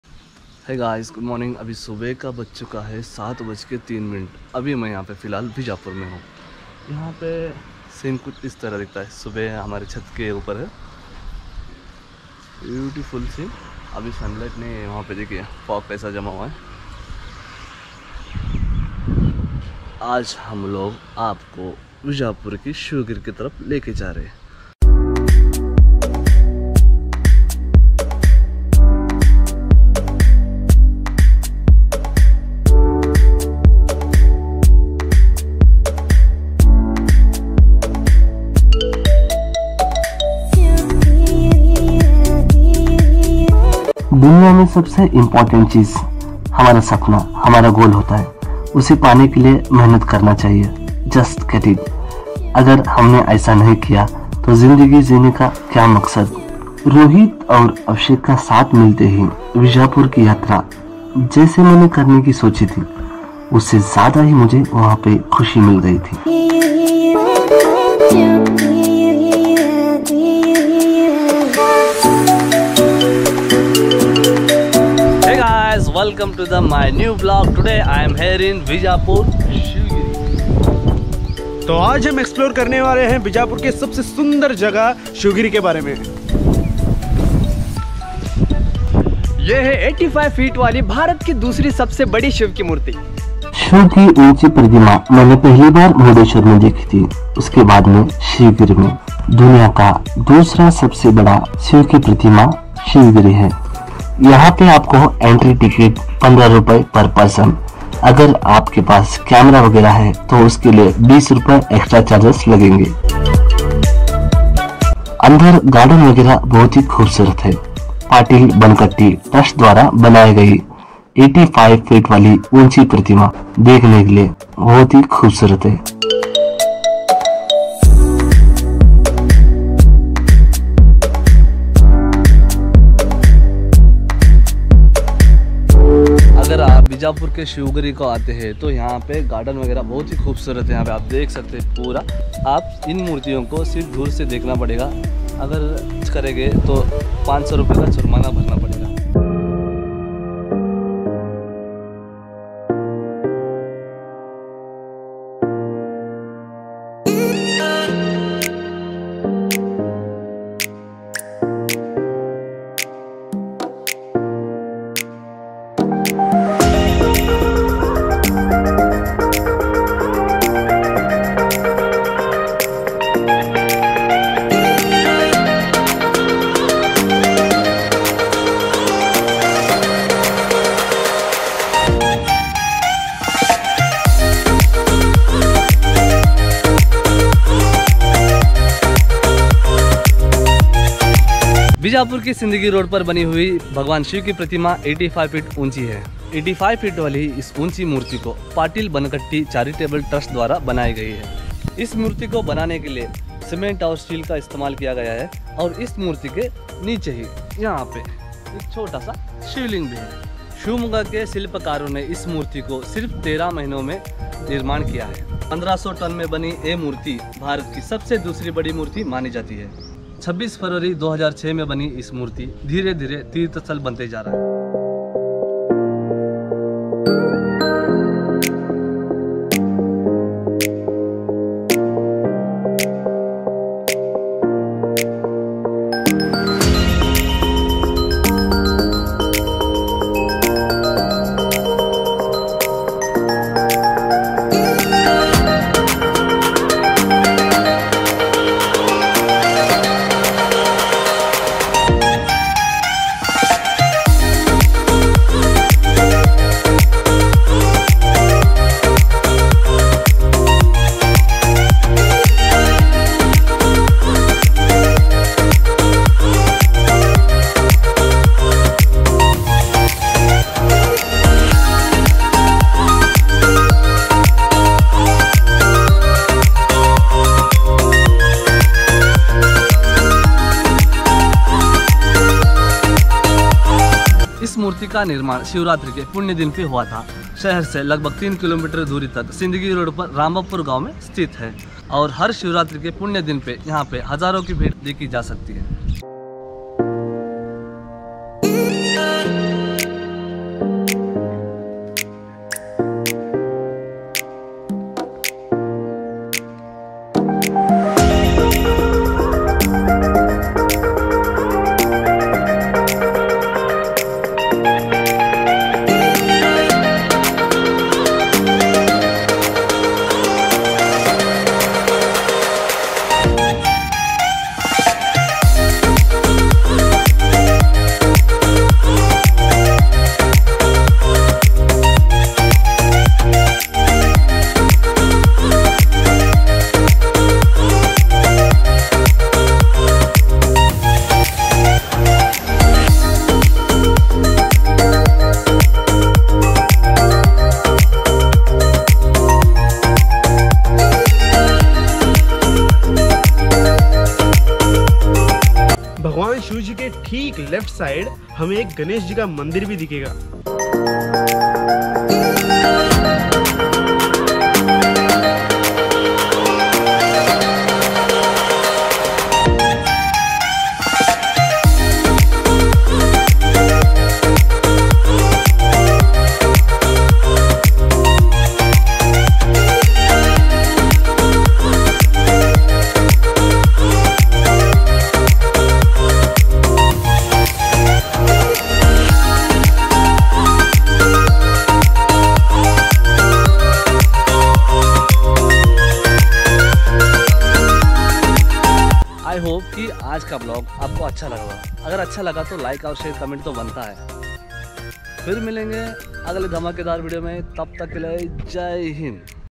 Bijapur Shivagiri vlog | 85 fit Lord Shiva statue Basant vana ukkali road Karnataka vlog
Hi friends this Bijapur (vijaypur) shivagiri 85 fit Shiva statue ukkali road Karnataka it's very beautiful statue India's second largest Shiva statue , it's make shiva statue patil bankatti charitable trust